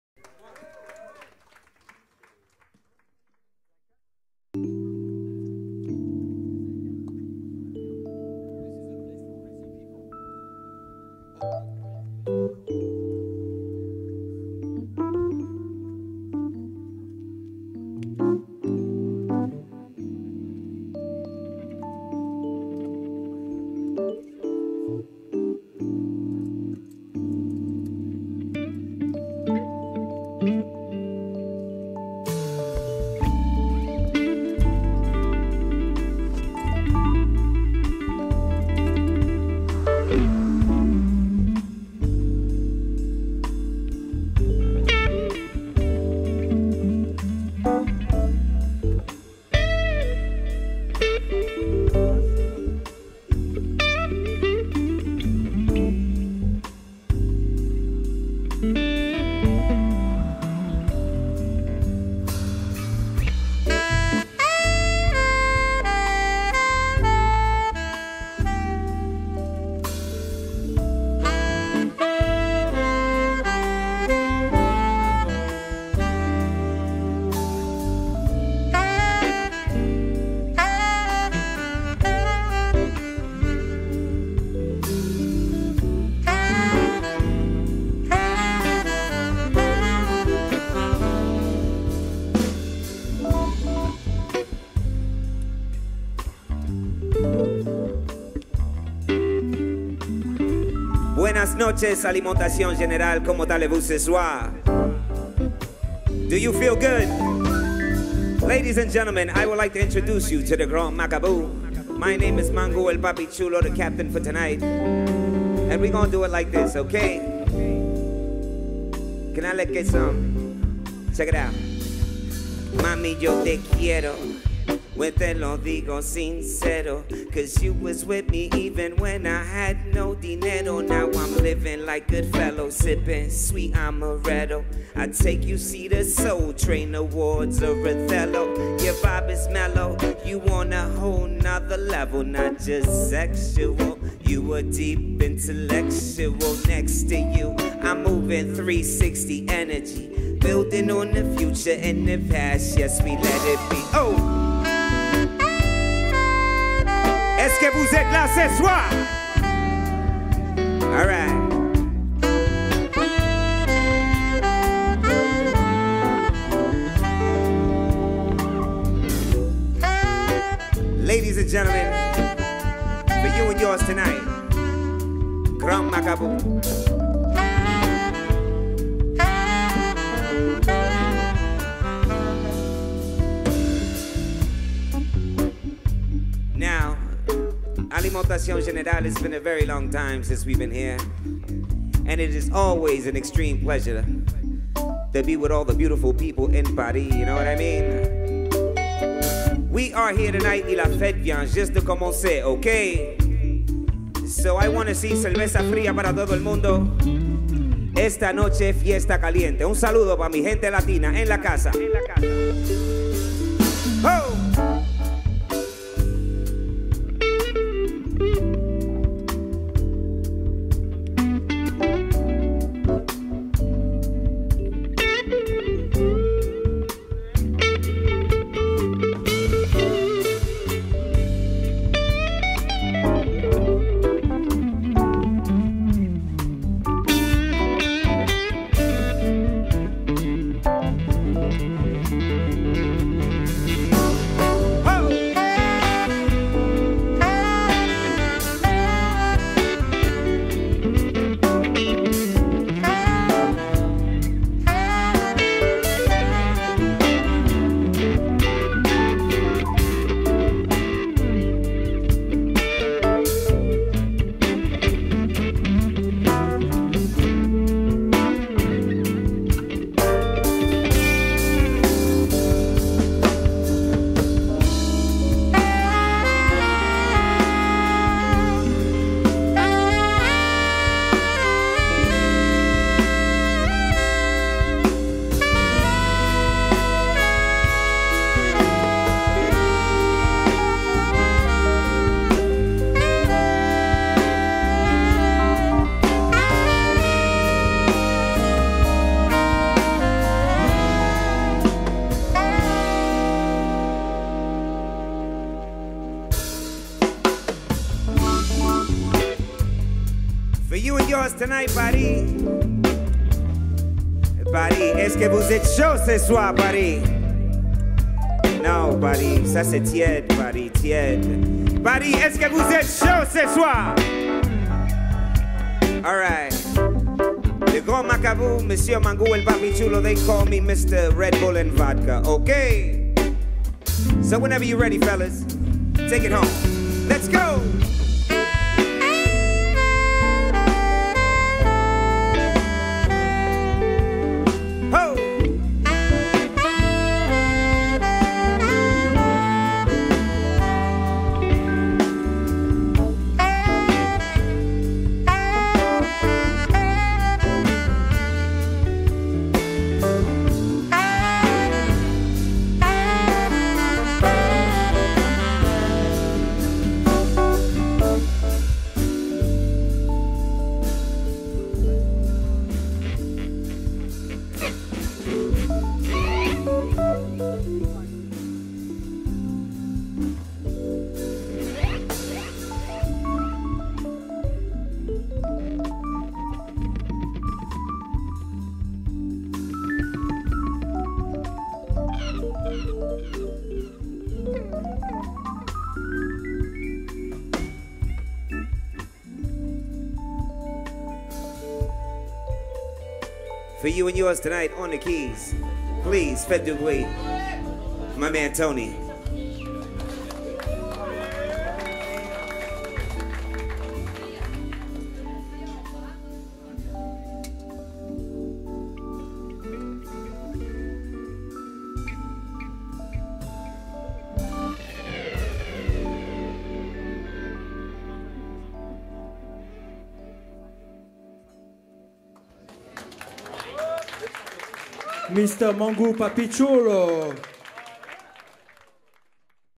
Le Grand Macabout (2) (novembre 2013) à l'alimentation Générale (Paris XI)
Captation live du Grand Macabout à l'Alg. nov. 2013)